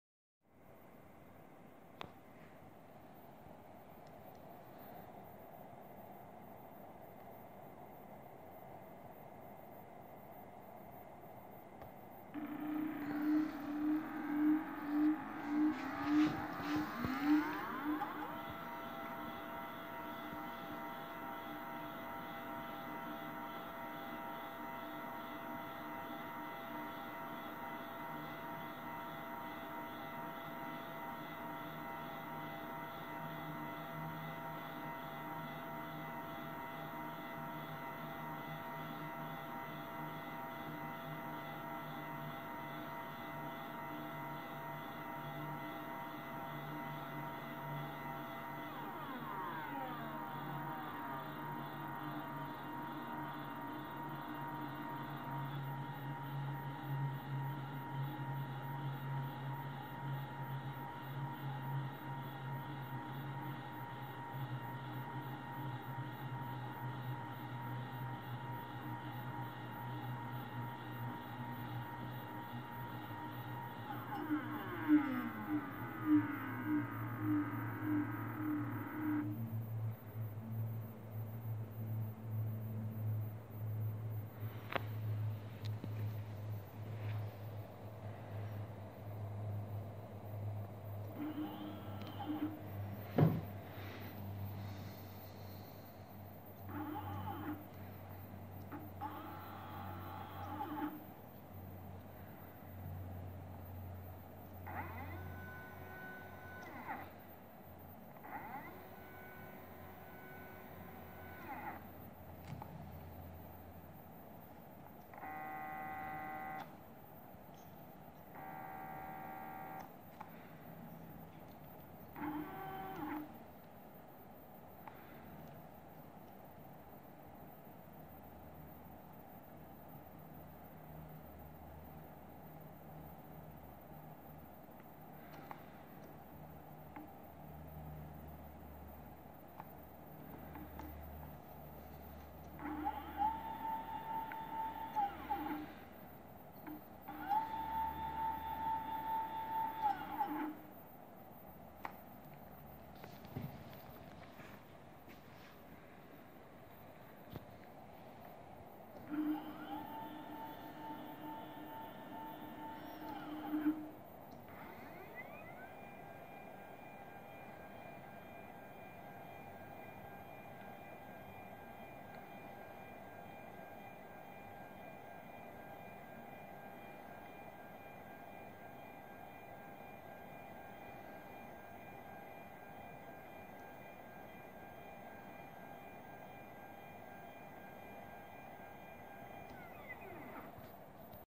Picastro A1 + moteurs Nanotec
Motorisation pour monture de télescope de fabrication personnelle.